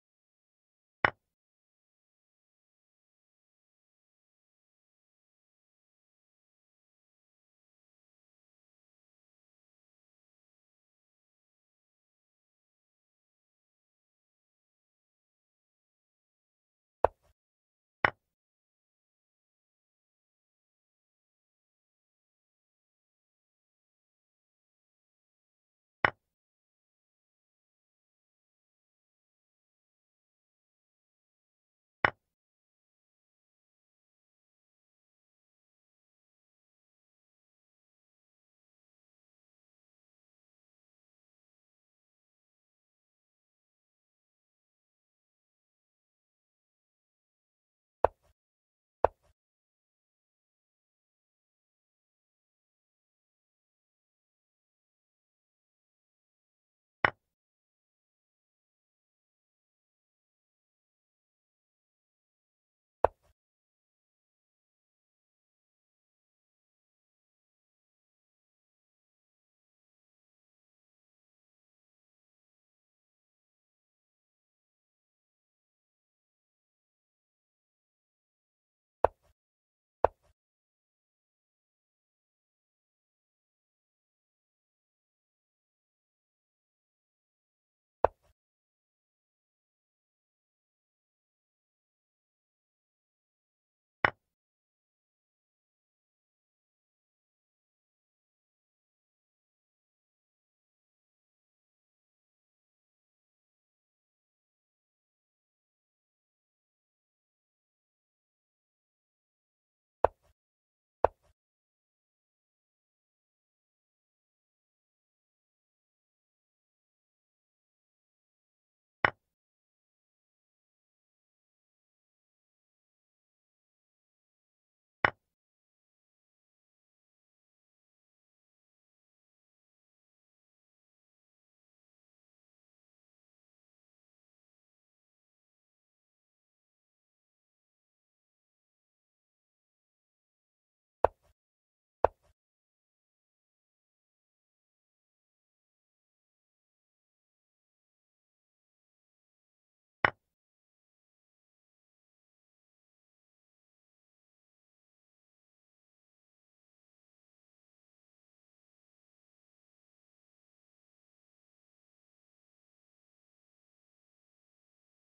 Chess Puzzles to Practice. Themes: Back rank mate, Short puzzle, Checkmate. Learn Chess
✅ PUZZLE THEMES
Puzzle 1
• Back rank mate: Checkmate the king on the home rank, when it is trapped there by its own pieces.
Puzzle 2
Puzzle 3
• Double check: Checking with two pieces at once, as a result of a discovered attack where both the moving piece and the unveiled piece attack the opponent's king.
• Mate in 2: Deliver checkmate in two moves.
Puzzle 4
• Middlegame: A tactic during the second phase of the game.
• Short puzzle: Two moves to win.